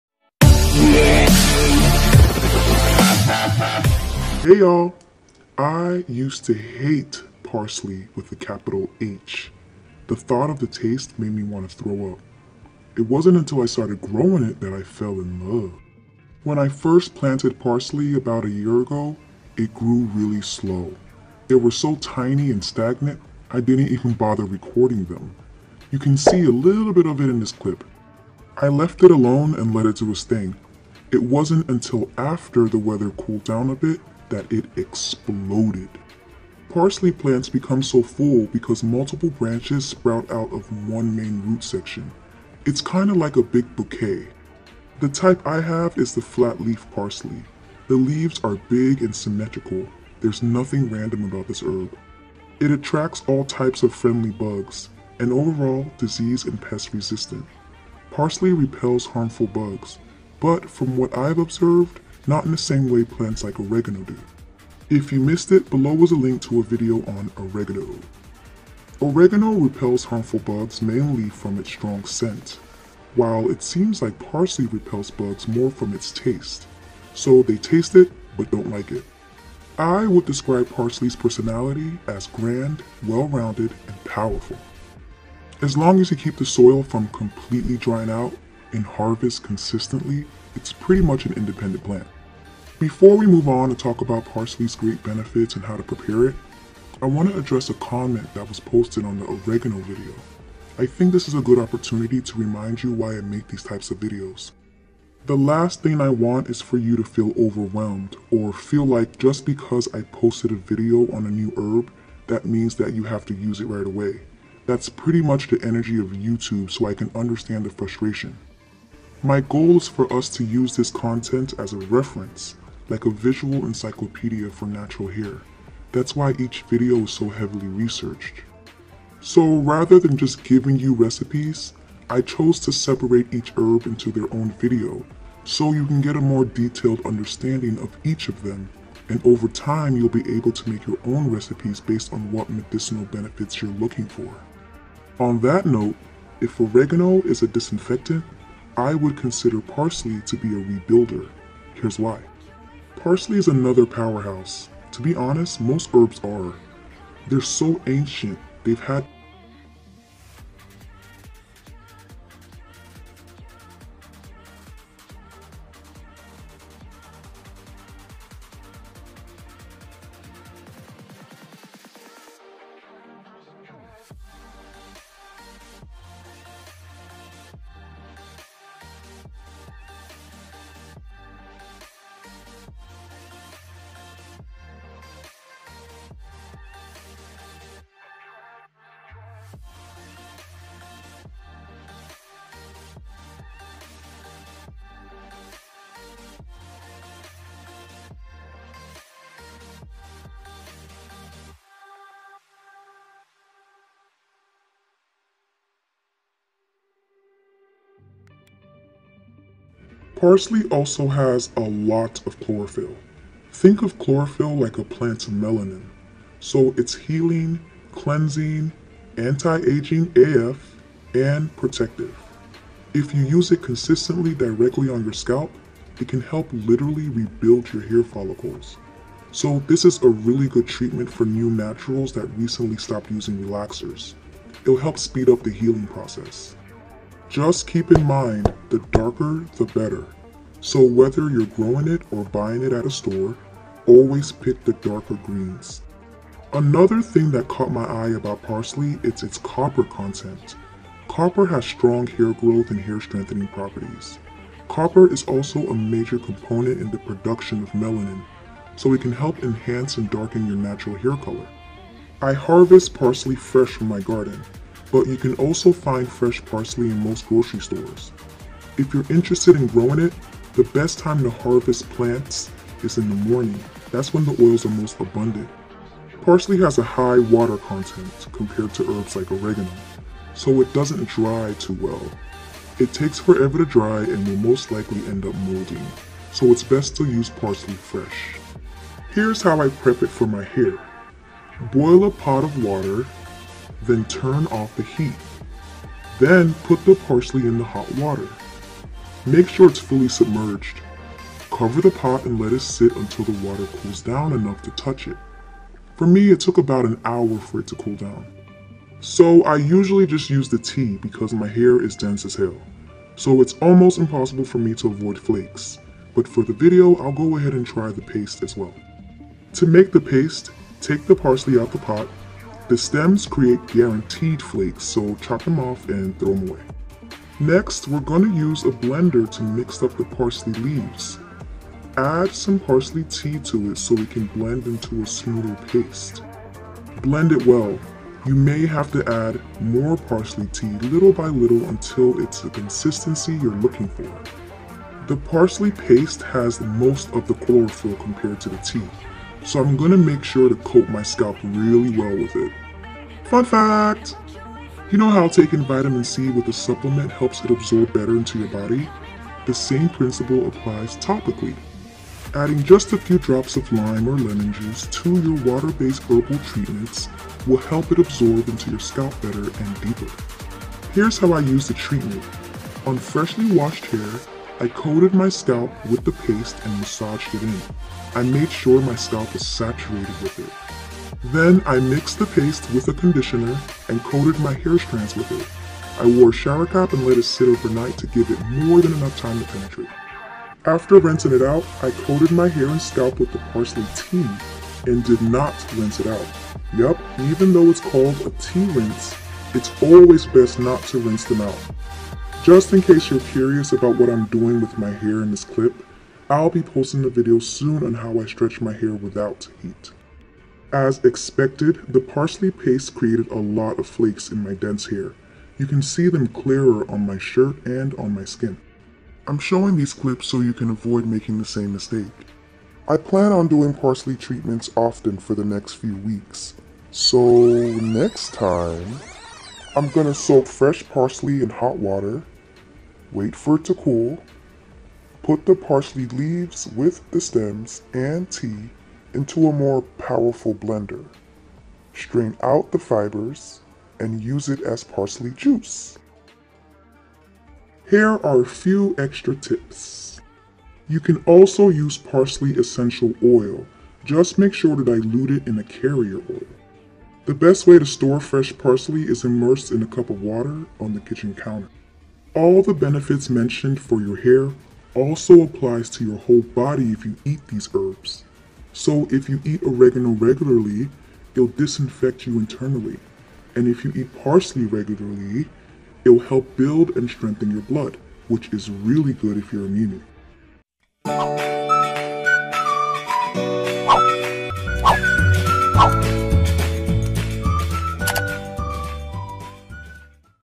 PARSLEY REBUILD YOUR HAIR | REMEDY TO GROW YOUR HAIR!
Parsley rebuilds and strengthens everything; your hair follicles, hair strands and if you eat it … your blood.

Best time to use 💡
o before and after wearing braids
o new natural
o if you use chemicals
o if your hair is thinning and/or becoming sparse
o help wake sleeping hair follicles – THINNING EDGES !!

REFERENCES 🔬📗

Effects of Parsley (Petroselinum crispum) and its Flavonol Constituents, Kaempferol and Quercetin, on Serum Uric Acid Levels, Biomarkers of Oxidative Stress and Liver Xanthine Oxidoreductase Activity inOxonate-Induced Hyperuricemic Rats

Antimicrobial effects of pepper, parsley, and dill and their roles in the microbiological quality enhancement of traditional Egyptian Kareish cheese

Chemical Composition, Antioxidant and Antimicrobial Activity of Essential Oils from Organic Fennel, Parsley, and Lavender from Spain

The Antibacterial Activity of Aqueous Extraction of Petroselinum crispum (Parsley) and Rhus Glabra (Smooth Sumac)

Topical Apigenin Improves Epidermal Permeability Barrier Homeostasis in Normal Murine Skin by Divergent Mechanisms

A cell-based system for screening hair growth-promoting agents.